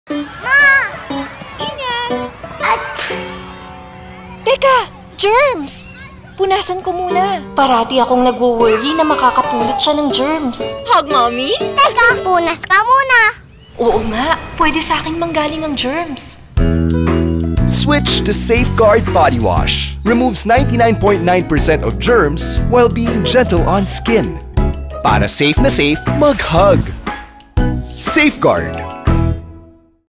Safe Hugs with Safeguard Bodywash
Spread the Love, Not the Germs with Safeguard Bodywash. Removes 99.9% of Germs While Being Gentle on Skin. Basta Safeguard, Safe!